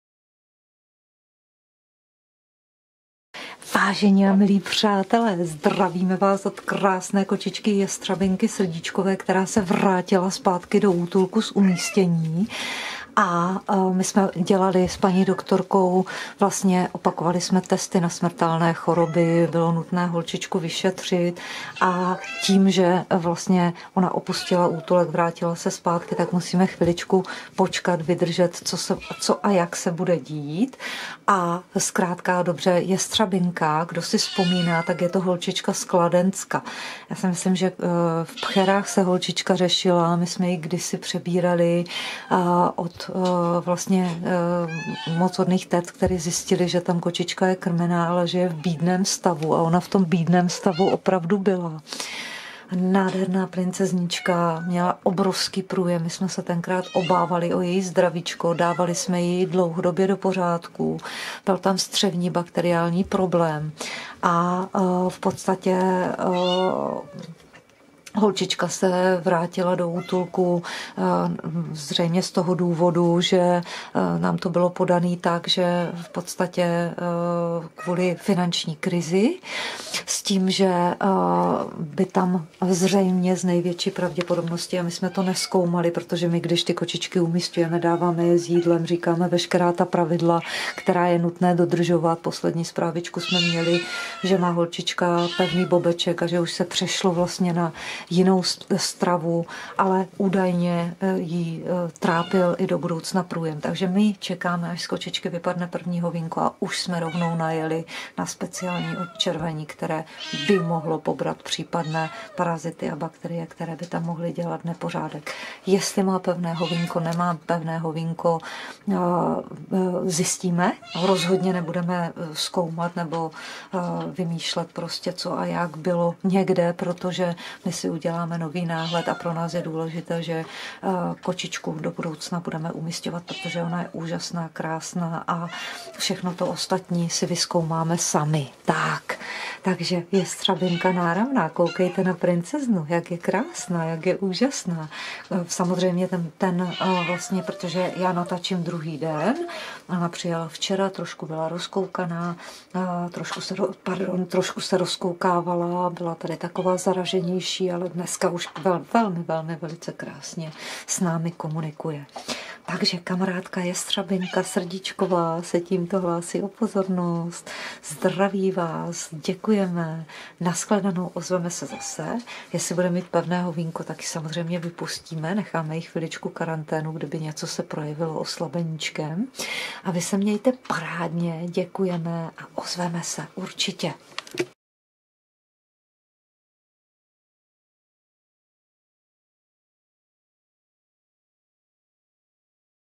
Jestřabinka (22.1.2021)
Mazlivá kráska Jestřabinka Srdíčková se před pár dny vrátila z umístění. Níže kočička kopíruje vstupní text a dnes přidává video pozdrav z hospitalizačního boxíčku Koukněte na krásnou Jestřabinku, které hledáme nový domov v bytových podmínkách. Držíme tlapičky, přejeme pevné zdravíčko a jedeme dál.
"Krásná mladá princezna Jestřabinka Srdíčková se po devíti měsících vrátila z umístění zpět do útulku kvůli finanční situaci v rodině Před návratem do útulku jsme dostali informaci, že měla holčička měkčí bobeček a užívá probiotika. Jestřabinka je zpátky v útulku v srdíčkové péči, proto bychom byli rádi, kdyby nedocházelo ke zbytečným negativním komentářům. Zpátky k Jestřabince, kočička přišla před časem do útulku vyhublá, nemocná a s průjmem. Zjistili jsme střevní bakteriální problém, který jsme časem zdárně překonali. Po návratu z nového domova máme holčičku v hospitalizačním boxíku, pořídili jsme s paní doktorkou MVDr. Veronikou Kadlecovou vstupní vyšetření, opakované testy na FIV/FeLV a nyní budeme sledovat kakání. Milovaná Jestřabinka dostala rovnou speciální odčervení a uvidíme, co nám přinese čas. Rozhodně jí budeme hledat nový domov v bytových podmínkách u prima páníčků a případného zvířecího kamaráda. Tak zatím pac a pusu, Jestřabinka se vám brzy opět ozve."